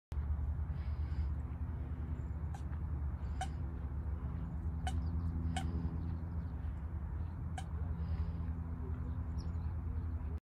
The American Coot!
This video shows an American Coot who is swimming in a freshwater marsh habitat.

"Coots are tough, adaptable waterbirds. Although they are related to the secretive rails, they swim in the open like ducks and walk about on the shore, making themselves at home on golf courses and city park ponds. They are usually in flocks, they are aggressive and noisy, making a wide variety of calls by day or night. They have strong legs and big feet with lobed toes, and coots fighting over territorial boundaries will rear up and attack each other with their feet. Often seen walking on open ground near ponds. In taking flight they must patter across the water, flapping their wings furiously, before becoming airborne." - Audubon.org